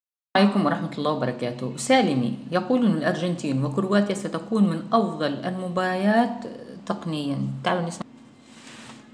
هذا ماقاله المحللين/كرواتيا والارجنتين تكون مباراتهم اروع المباريات تقنيا
يوميات_شاديه_العموري اسمع ماقاله مباراة الأرجنتين وكرواتيا اروع مباراة تقنيا